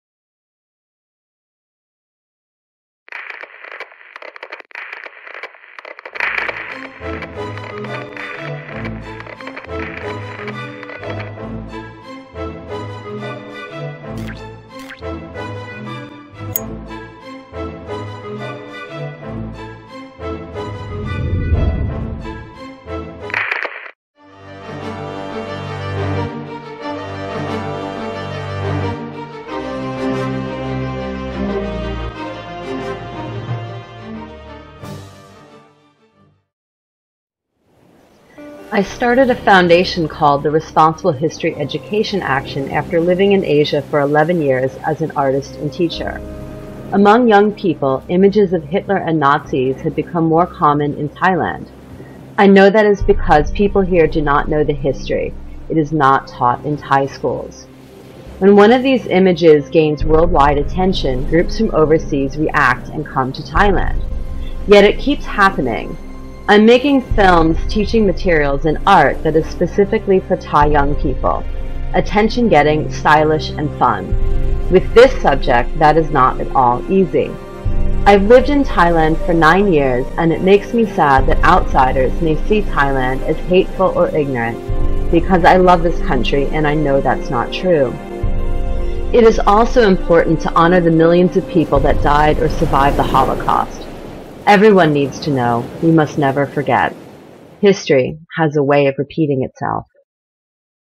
The Kickstarter Video for Our Project
UPDATE: We are working on an animated version of this project thanks to a generous donation of professional videography services from a supporter!  We will post that version on youtube December 2014.

We're raising money to fund a public service announcement type TV ad, but a very non-traditional one.  This was shot in Rayong and the audio needs work, but this is the almost-final, final version.